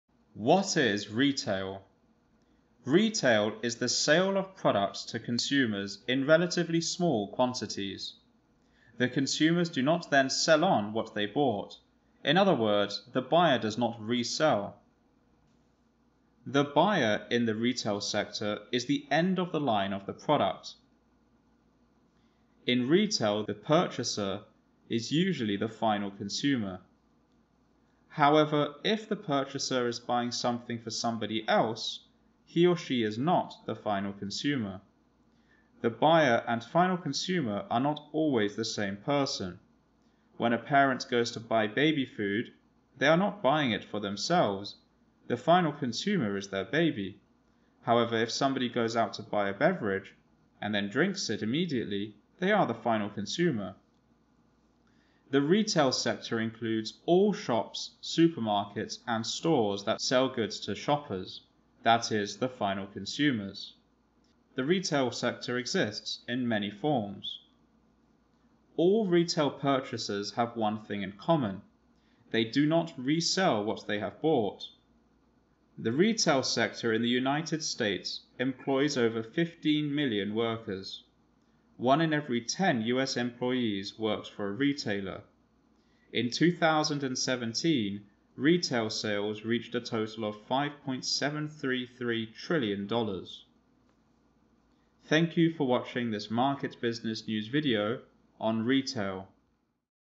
What is Retail?
This video explains what retail is. Retail refers to the sale of goods to ultimate consumers.